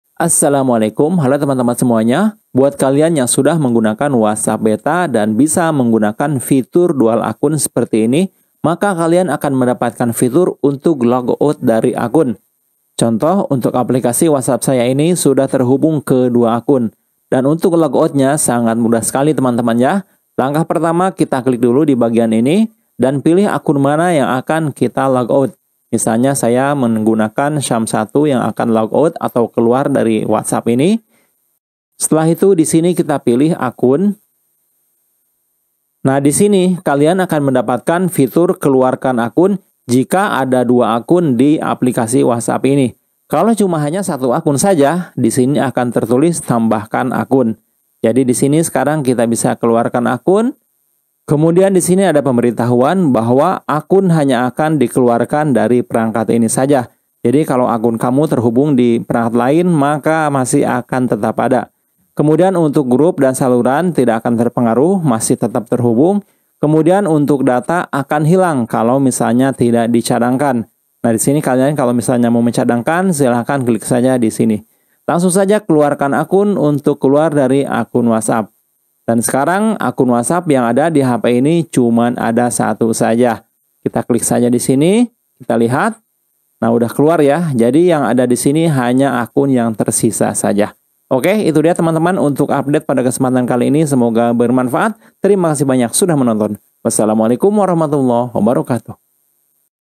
Cara Keluar Akun Whatsapp Langsung Dari Aplikasinya
Assalamualaikum apa kabar teman-teman semuanya, beberapa Minggu yang lalu WhatsApp sudah meluncur fitur baru yaitu dual akun dalam satu aplikasi.
Selain meluncurkan fitur tersebut,ternyata WhatsApp juga menambahkan fitur tambahan baru lainnya,yaitu logout atau keluar dari akun yang sedang kita gunakan.

Fitur tersebut baru hadir pada aplikasi WhatsApp Beta yang sudah memiliki fitur dual akun,nah buat kalian yang mau mencobanya silahkan update terlebih dahulu WhatsApp yang kalian gunakan ke WhatsApp Beta terbaru.
Caranya silahkan kalian download aplikasi WhatsApp Beta pada link yang sudah saya sediakan di bawah, setelah itu install dan untuk melakukan update ke WhatsApp Beta.
Aplikasi ini aman karena merupakan aplikasi resmi dari WhatsApp yang sama sekali belum pernah dimodifikasi.

Terima Kasih